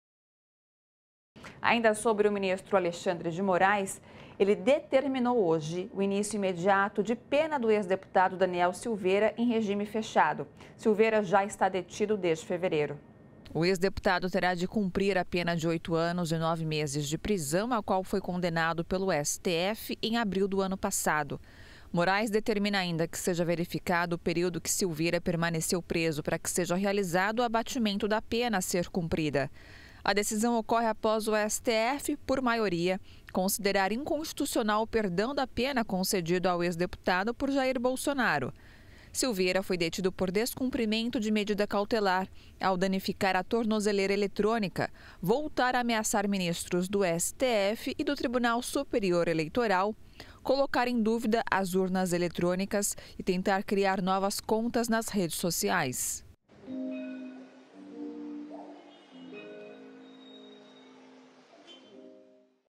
Moraes manda Silveira cumprir pena em regime fechado
O ministro Alexandre de Moraes determinou o início imediato de pena do ex-deputado Daniel Silveira em regime fechado. Silveira já está detido desde fevereiro por descumprimento de medida cautelar.